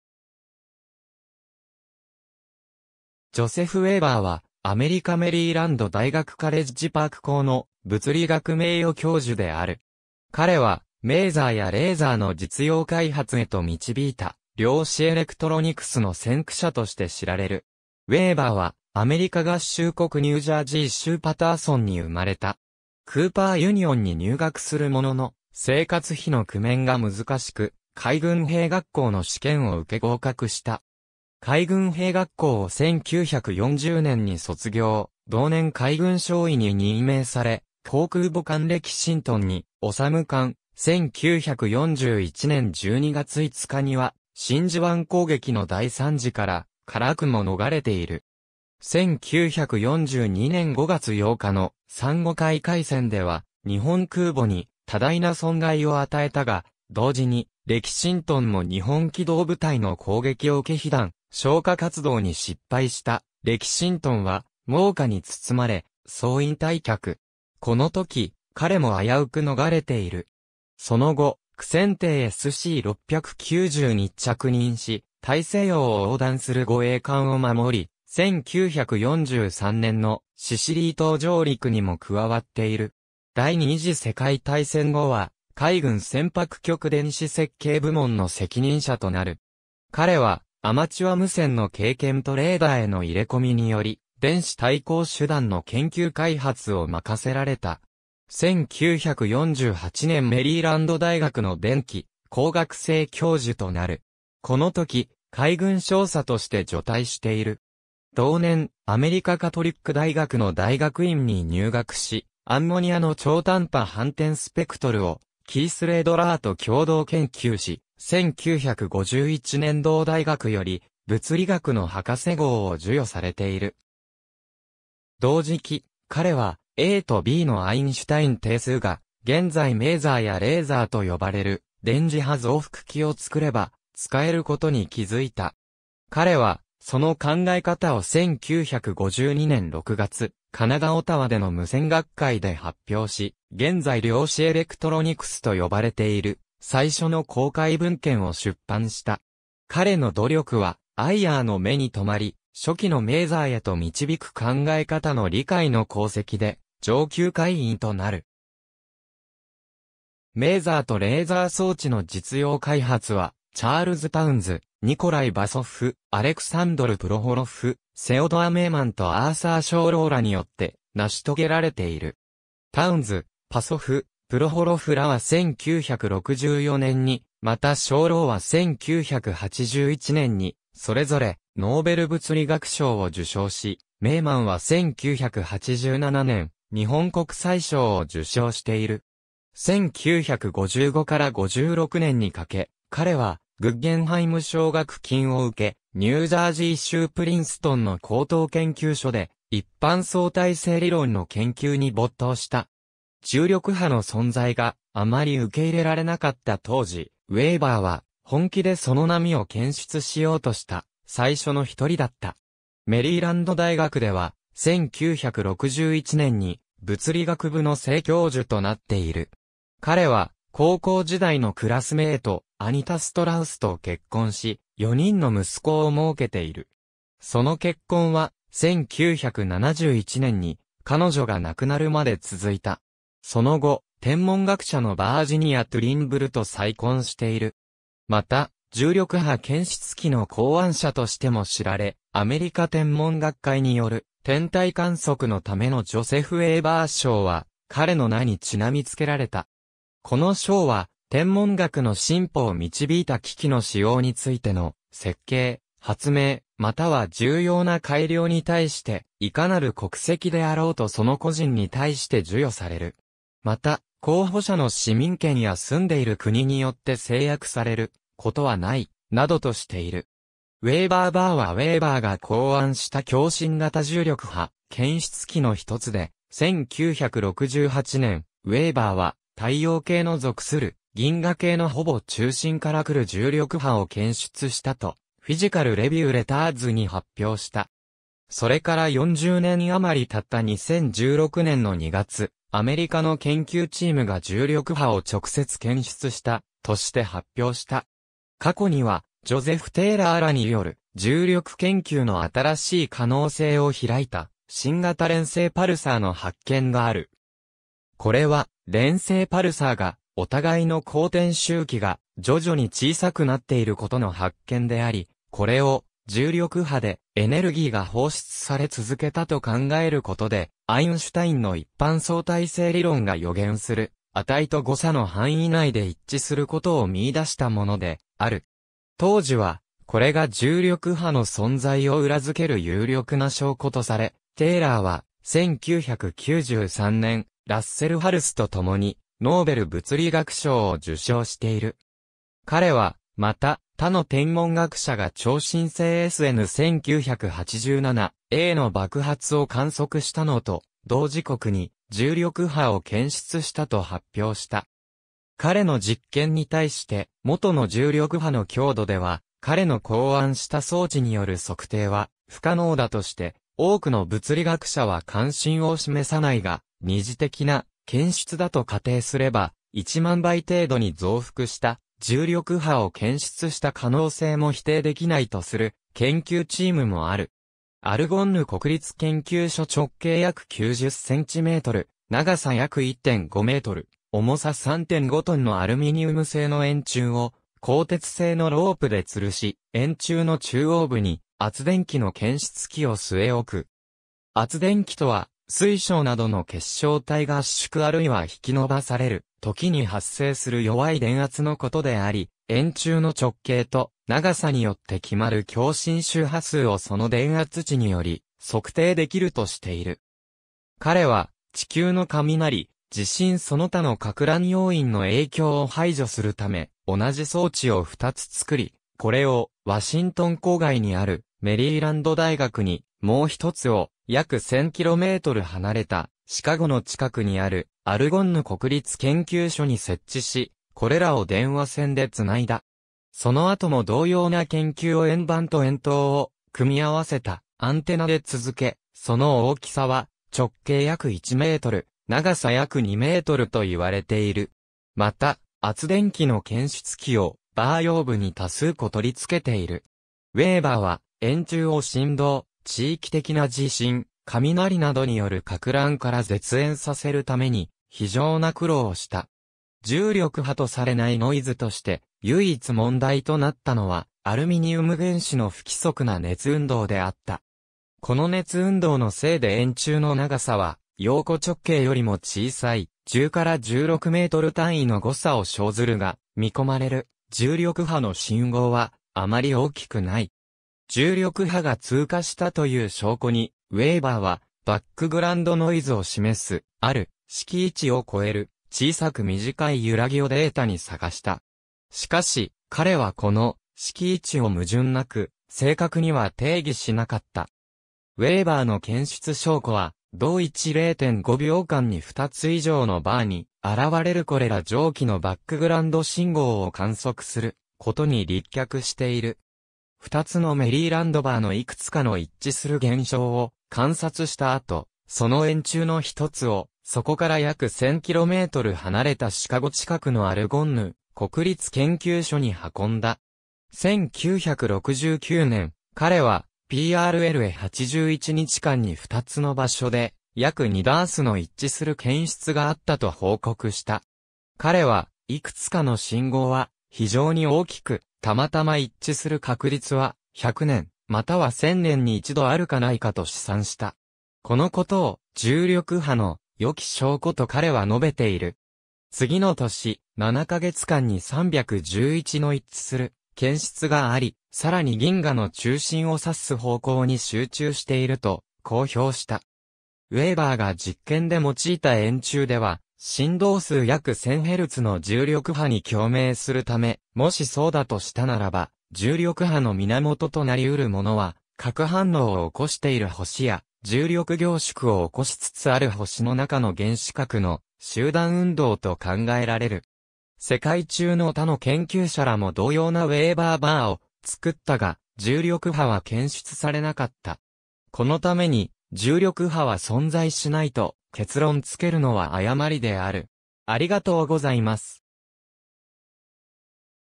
ジョセフ・ウェーバー
ジョセフ・ウェーバー（Joseph Weber、1919年5月17日 - 2000年9月30日）は、アメリカメリーランド大学カレッジパーク校の物理学名誉教授である。
彼は、メーザーやレーザーの実用開発へと導いた量子エレクトロニクスの先駆者として知られる。
ウェーバーは、アメリカ合衆国ニュージャージー州パターソンに生まれた。
クーパー・ユニオンに入学するものの、生活費の工面が難しく、海軍兵学校の試験を受け合格した。
海軍兵学校を1940年に卒業、同年海軍少尉に任命され、航空母艦レキシントンに就艦、1941年12月5日には、真珠湾攻撃の大惨事から、からくも遁れている。
1942年5月8日の珊瑚海海戦では、日本空母に多大な損害を与えたが、同時にレキシントンも日本機動部隊の攻撃を受け被弾、消火活動に失敗した「レキシントン」は、猛火に包まれ、総員退却。
この時、彼も危うく逃れている。
その後、駆潜艇SC690に着任し、大西洋を横断する護衛艦をまもり、1943年のシシリー島上陸にも加わっている。
第二次世界大戦後は、海軍船舶局電子設計部門の責任者となる。
彼は、アマチュア無線の経験とレーダーへの入れ込みにより、電子対抗手段(ECM)の研究開発を任せられた。
1948年メリーランド大学の電気工学正教授となる。
この時、海軍少佐として除隊している。
同年、アメリカカトリック大学の大学院に入学し、アンモニアの超短波反転スペクトルをキース・レイドラーと共同研究し、1951年同大学より物理学の博士号（Ph.D.）を授与されている。
同時期、彼はAとBのアインシュタイン定数が、現在メーザーやレーザーと呼ばれる電磁波増幅器を作れば、使えることに気づいた。
彼は、その考え方を1952年6月、カナダ・オタワでの無線学会(IRE)で発表し、現在量子エレクトロニクスと呼ばれている最初の公開文献を出版した。
彼の努力はIRE（現IEEE）の目に留まり、「初期のメーザーへと導く考え方の理解」の功績で上級会員となる（1958年）。
メーザーとレーザー装置の実用開発は、チャールズ・タウンズ、ニコライ・バソフ、アレクサンドル・プロホロフ、セオドア・メイマンとアーサー・ショーローらによって成し遂げられている。
タウンズ、パソフ、プロホロフらは1964年に、またショーローは1981年に、それぞれノーベル物理学賞を受賞し、メイマンは1987年、日本国際賞を受賞している。
1955～56年にかけ、彼は、グッゲンハイム奨学金を受け、ニュージャージー州プリンストンの高等研究所で、一般相対性理論の研究に没頭した。
重力波の存在が、あまり受け入れられなかった当時、ウェーバーは、本気でその波を検出しようとした最初の一人だった。
メリーランド大学では、1961年に物理学部の正教授となっている。
彼は、高校時代のクラスメイト、アニタ・ストラウスと結婚し、4人の息子をもうけている。
その結婚は、1971年に彼女が亡くなるまで続いた。
その後、天文学者のバージニア・トゥリンブルと再婚している。
また、重力波検出器（ウェーバー・バー）の考案者としても知られ、アメリカ天文学会による天体観測のためのジョセフ・ウェーバー賞は、彼の名に因み付けられた。
この賞は、天文学の進歩を導いた器機の使用（ソフトウェアではなく）についての設計、発明または重要な改良に対して、いかなる国籍であろうとその個人に対して授与される。
また、候補者の市民権や住んでいる国によって制約されることはない、などとしている。
ウェーバー・バーはウェーバーが考案した共振型重力波検出器の一つで、1968年、ウェーバーは太陽系の属する銀河系のほぼ中心から来る重力波を検出したとフィジカル・レビュー・レターズに発表した。
それから40年余りたった2016年の2月、アメリカの研究チームが重力波を直接検出した、として発表した。
過去にはジョゼフ・テイラーらによる「重力研究の新しい可能性を開いた新型連星パルサーの発見」がある。
これは連星パルサーがお互いの公転周期が徐々に小さくなっていることの発見であり、これを重力波でエネルギーが放出され続けたと考えることでアインシュタインの一般相対性理論が...